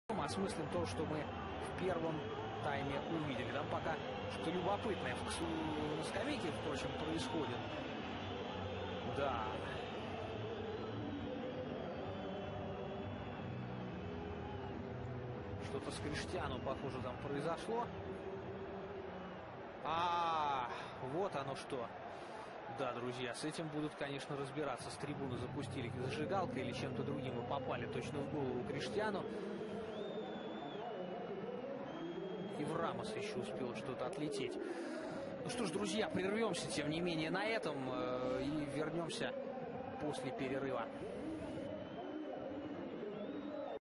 Ronaldo bisa frente ao Atletico e adepto atira com isqueiro com toda a força!
Cristiano Ronaldo está a ser a grande figura do dérbi de Madrid, entre o Atlético e o Real, com dois golos marcados, de grande penalidade.

 A verdade é que ao intervalo, os adeptos do Atlético estavam muito desagradados e quem sofreu as consequências foi o português.